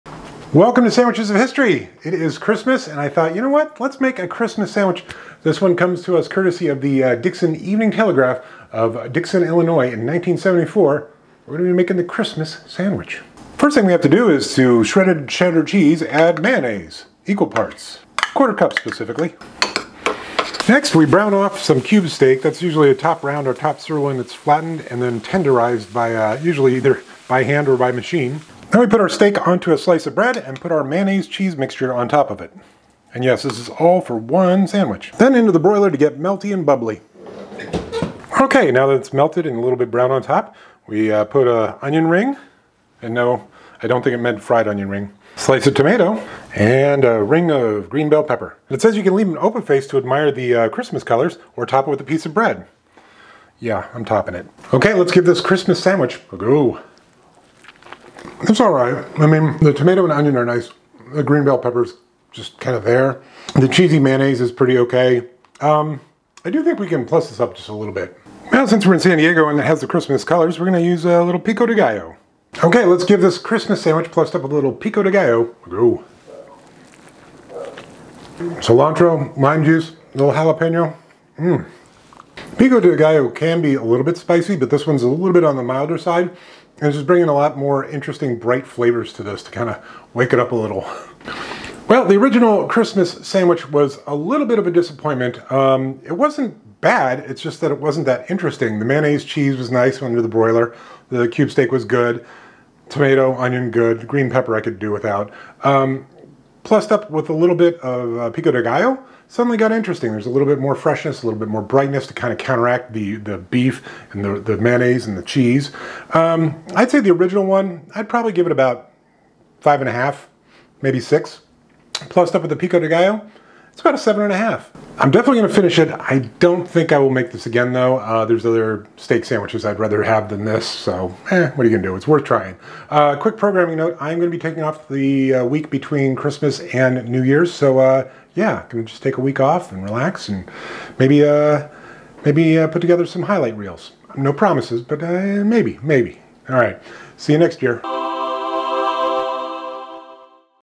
Christmas Sandwich (1974) on Sandwiches of History⁣
What better way to celebrate Christmas on Sandwiches of History than with a Christmas Sandwich. This recipe comes courtesy of the Dixon Evening Telegraph of 1974. What makes it a Christmas sandwich? Why the white onion ring, slice of tomato, and slice of green bell pepper, of curse. Duh! Lol Still, this could be tasty as it involves cube steak and cheddar cheese. As I mention at the end of the video, I am taking the week between Christmas and New Year’s Day off from posting sandwiches. I am hoping to maybe put together some other stuff to post but we’ll see. Thank you for following along on this journey of old sandwich recipes. Here’s to a happy, healthy, and hopefully more peaceful 2024.